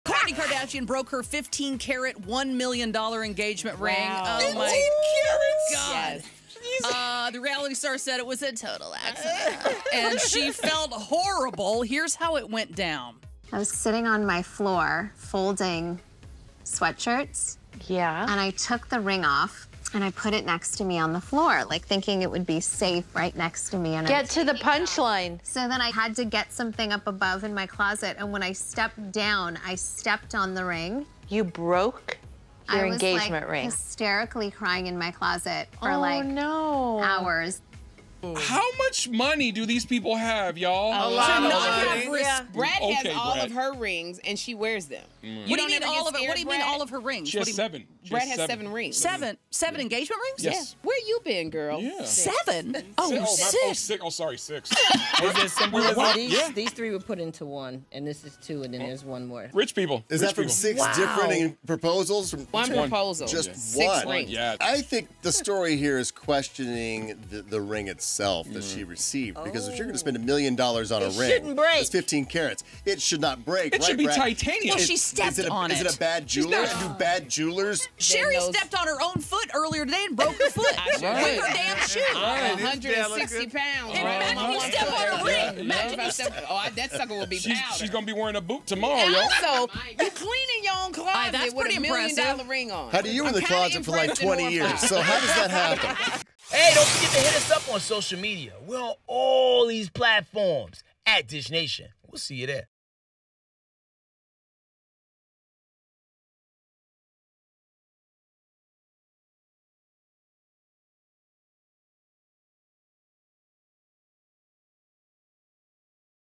Did KourtneyKardashian Just Break Her Engagement With TravisBarker?
Nah, Kourt is still good in the love department! She did accidentally break the  $1 million engagement ring her fiancé gave her though!

HOSTS
ATLANTA: